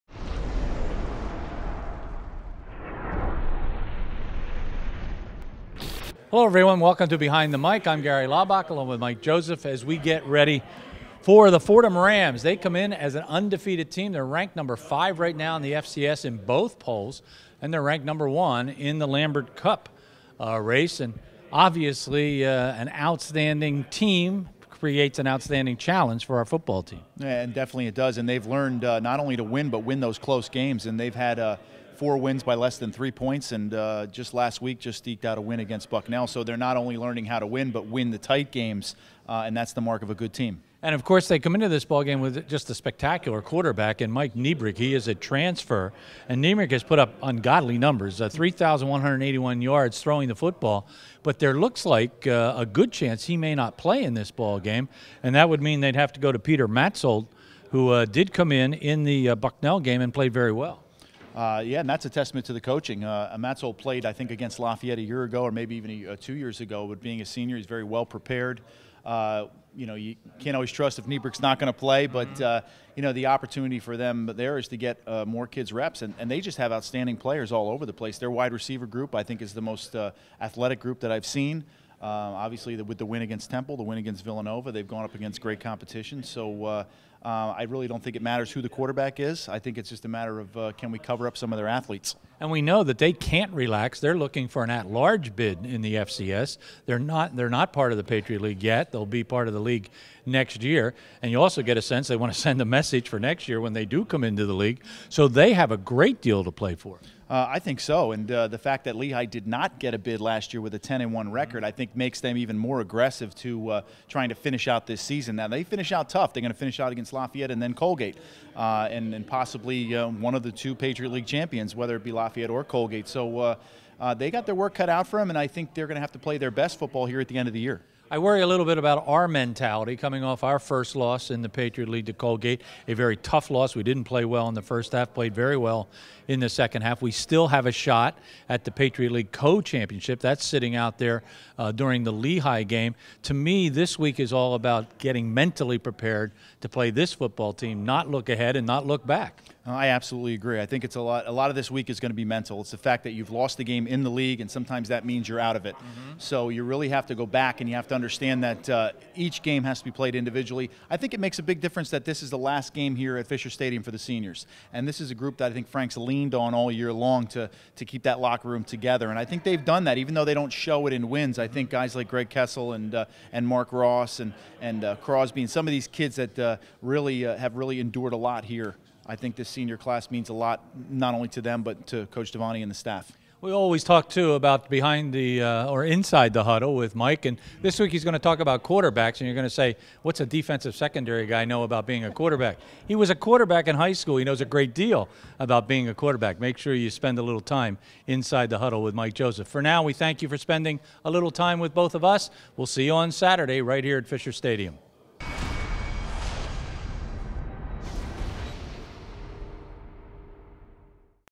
Behind the Mic: Lafayette Football vs Fordham
The Lafayette Sports Network's Gary Laubach and Mike Joseph preview Lafayette's upcoming football game against the undefeated Fordham Rams.